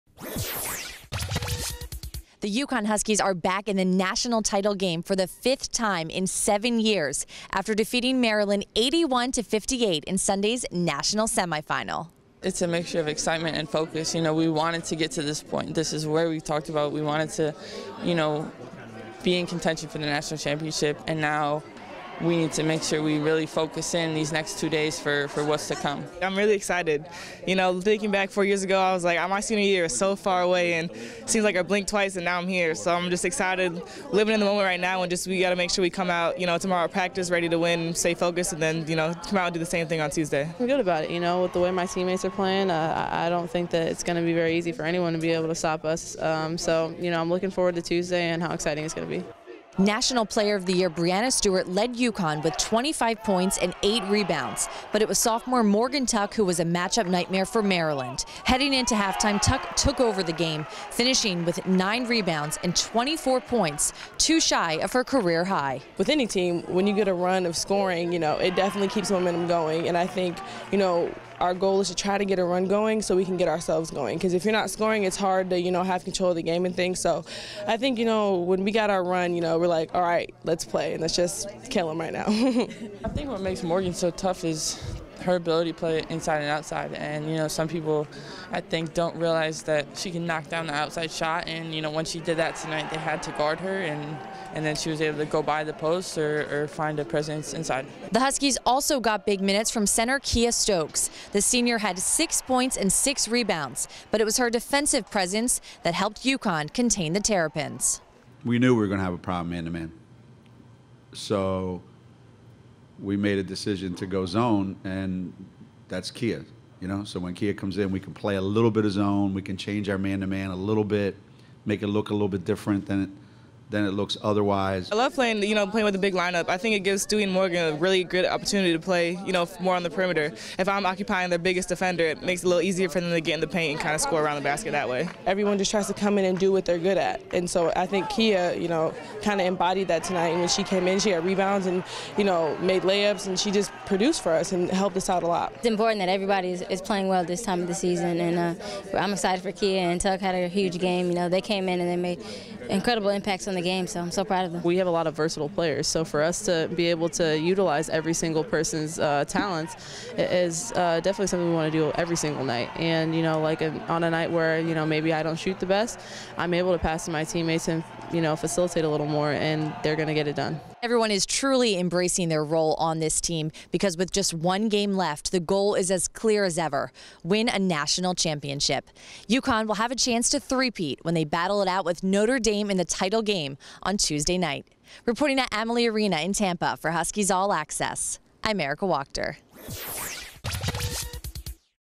UConn Back in NCAA Title Game with Win Over Maryland
Breanna Stewart and Morgan Tuck have UConn back in the championship game for the third straight year. Now the Huskies are one victory away from a third straight title.